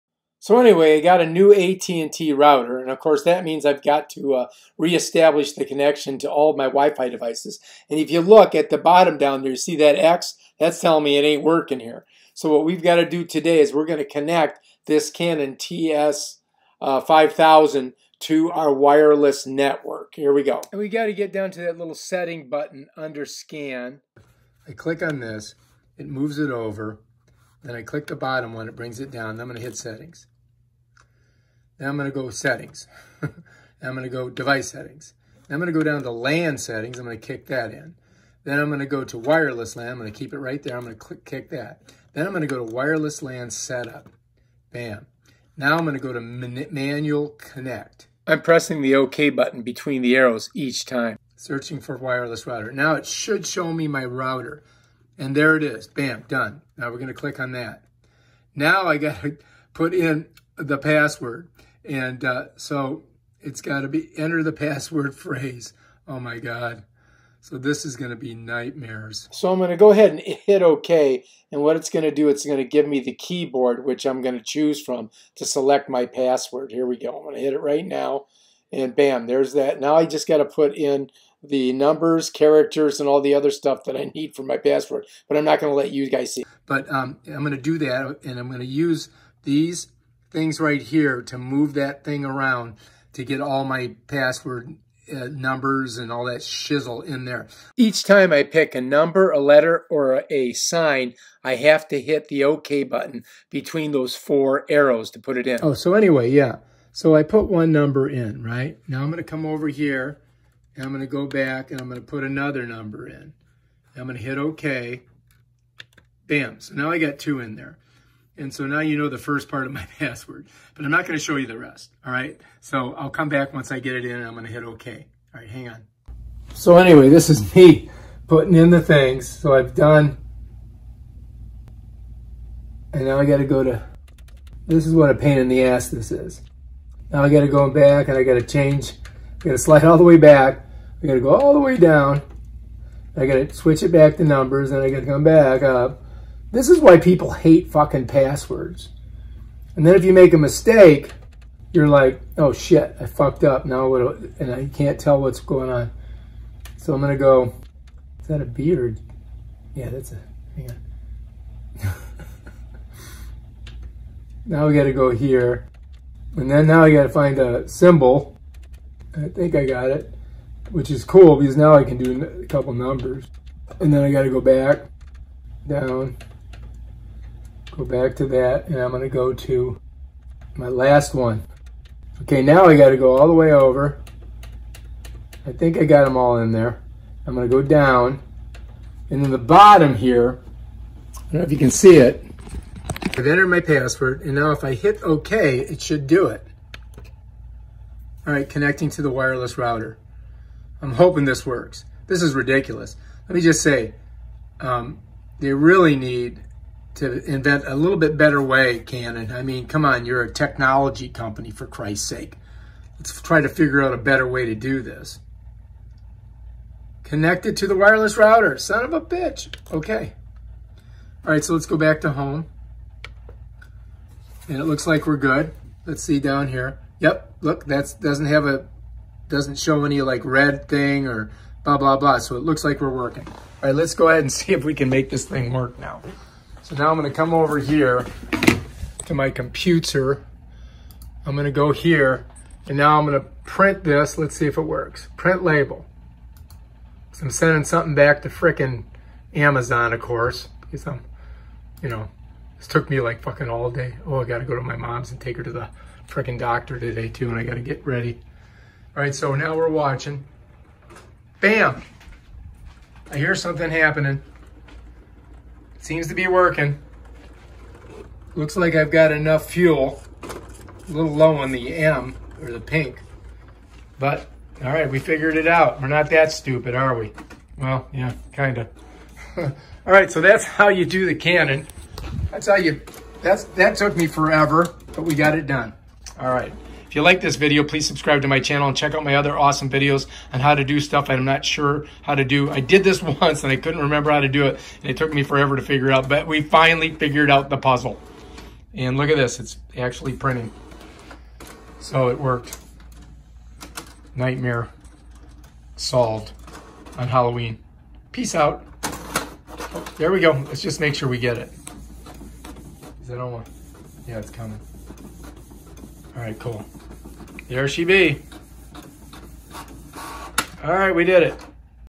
How to connect your Canon TS5000 printer to wifi network
How to connect your Canon TS5000 printer to wifi network. It's not so easy lots of keystrokes.